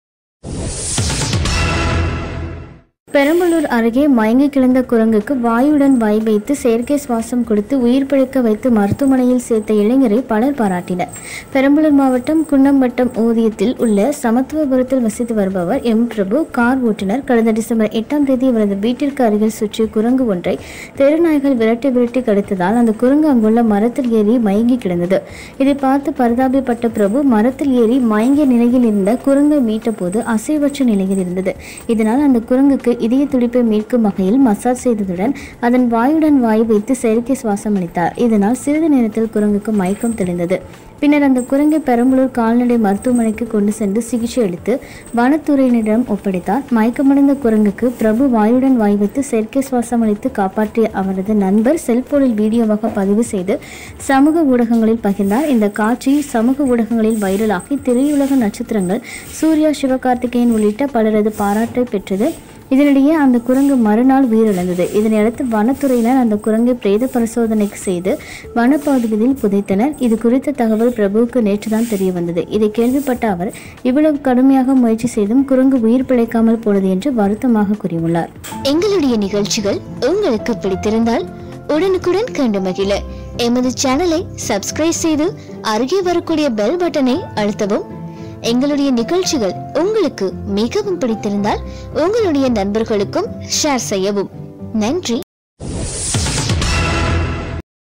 மயங்கிக் கிடந்த குரங்குக்கு செயற்கை சுவாசம் அளித்த இளைஞருக்கு குவியும் பாராட்டு- Tamil Voice
மயங்கிக் கிடந்த குரங்குக்கு செயற்கை சுவாசம் அளித்த இளைஞருக்கு குவியும் பாராட்டு